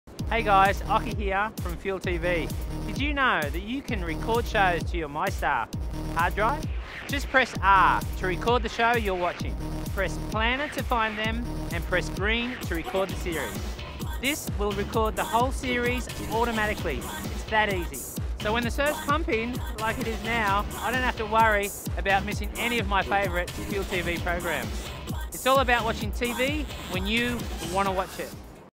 Occy from FUEL TV's MyStar tips - AUSTAR
FUEL TV host and Aussie surfing legend Mark "Occy" Occhilupo shares a tip for recording your favourite series to your MyStar on AUSTAR.